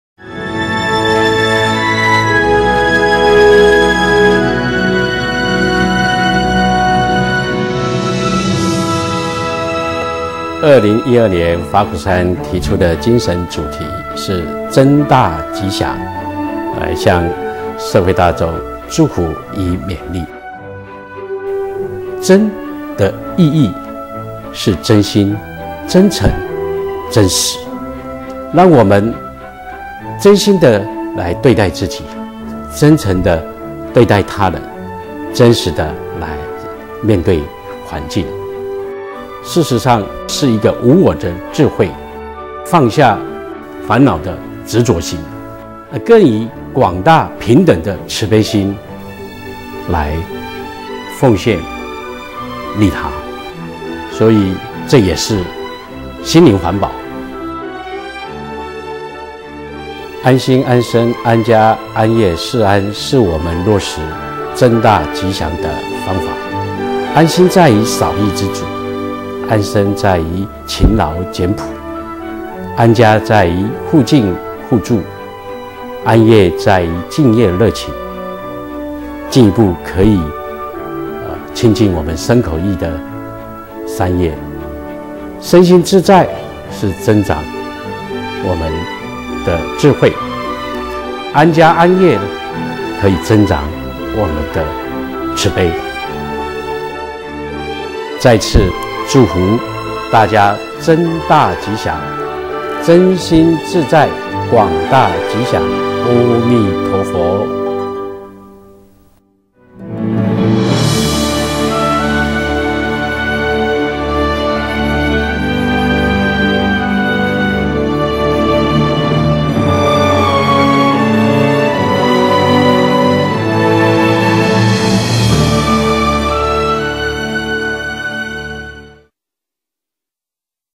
2012法鼓山─真大吉祥的祝福
2012年法鼓山提出的精神主題是真大吉祥
來向社會大眾祝福與勉勵
真 的意義
是真心 真誠 真實
讓我們真心的來對待自己
真誠的對待他人
真實的面對環境
事實上是一個無我的智慧
放下煩惱的執著心
更以廣大平等的慈悲心 來奉獻利他...